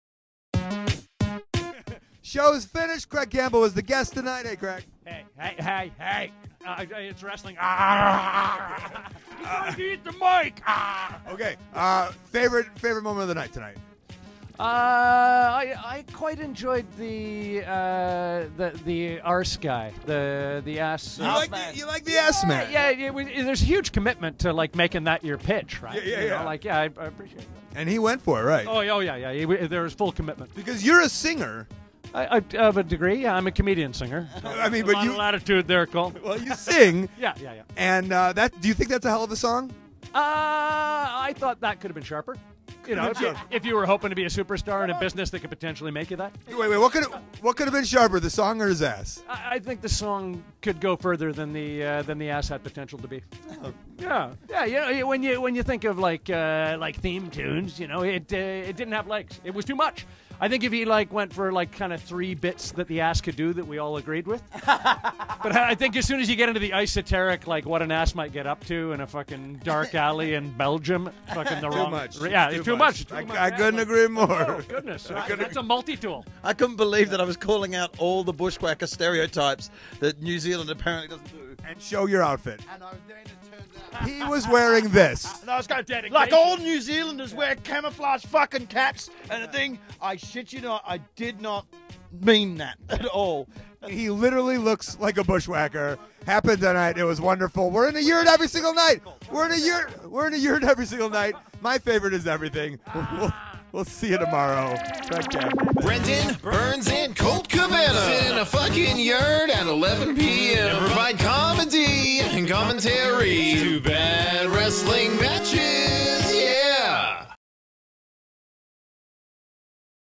2015 Ed Fringe RECAP Show #3 Colt Cabana & Brendon Burns w/ Craig Campbell
SHOW #3 - Saturday  Aug 8th
THE LIVE SHOW:
Brendon Burns & Colt Cabana sit in a F*cking Yurt at 11pm and do Comedy & Commentary to BAD WRESTLING MATCHES

OUR GUEST:
Craig Campbell
HIS LIVE SHOWS:
8:45pm "Craig Campbell: Don't Look Down"